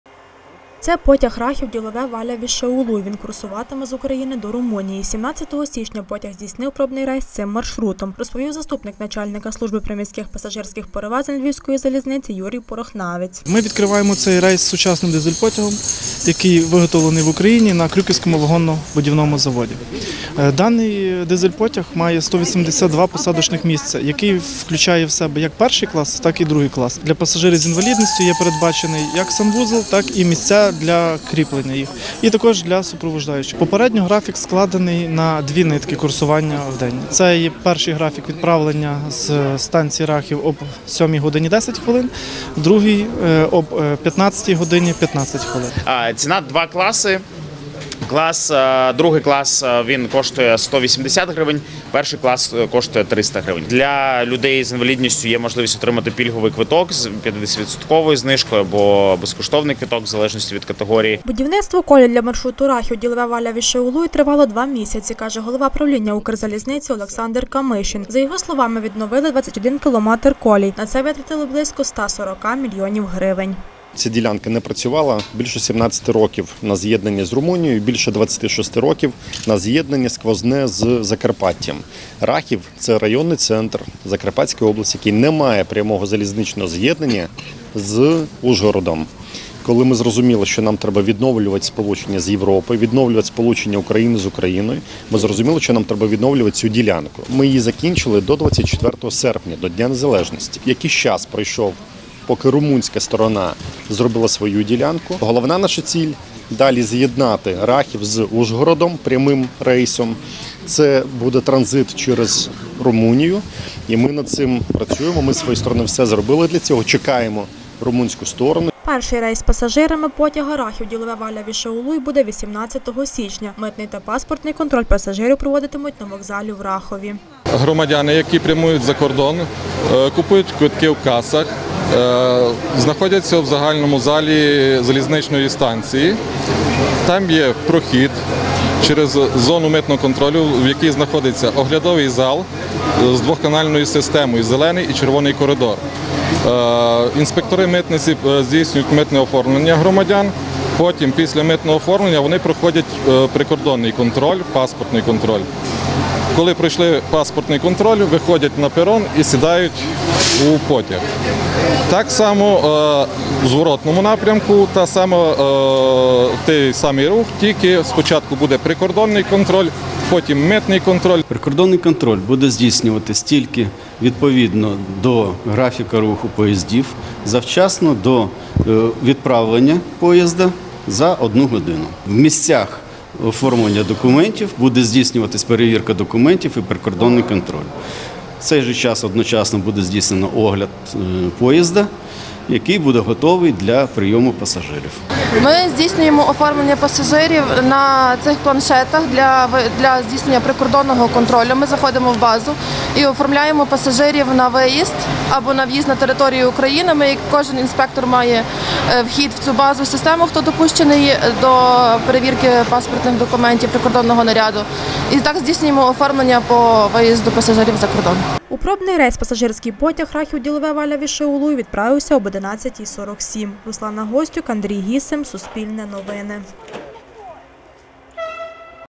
Потяг "Рахів – Ділове – Валя-Вішеулуй", який сполучає Україну з Румунією, здійснив пробний рейс
Пробний рейс здійснив 17 січня пасажирський потяг на ділянці "Рахів – Ділове – Валя-Вішеулуй", який сполучає Україну з Румунією. Перший рейс з пасажирами відправиться 18 січня, — розповів перший заступник начальника служби приміських пасажирських перевезень Львівської залізниці Юрій Порохнавець.

Потяг має три вагони й налічує 182 місця першого й другого класу. Ціна квитків 2 класу - 180 грн і 300 - першого. Також у ньому є місця для людей з інвалідністю та окрема вбиральня. Юрій Порохнавець додав, що гроші на потяг виділила "Укрзалізниця", загальна сума — 140 мільйонів гривень.